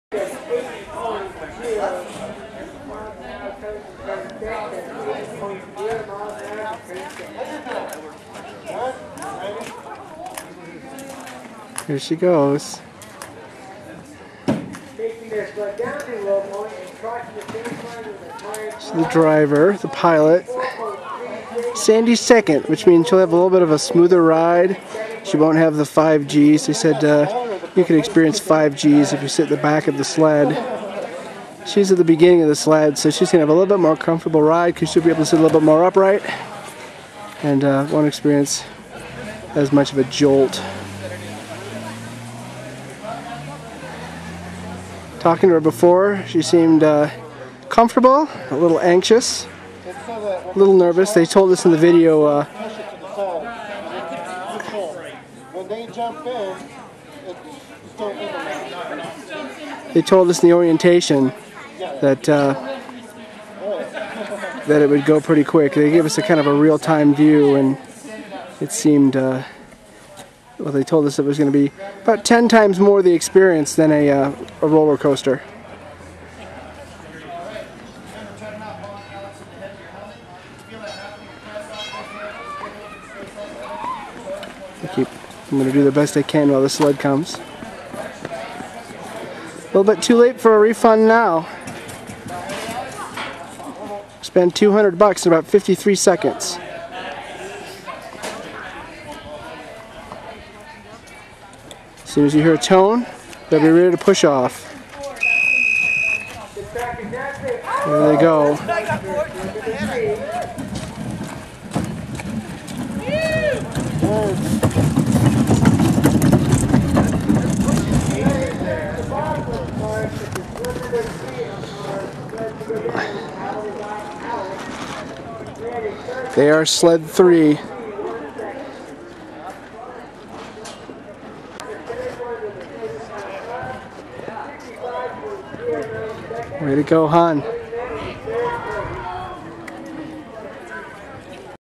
Olympics Part Deux - bobsled!!
me, a competition bobsleigh, the fastest North American bobsled track, 5G's...need I say more?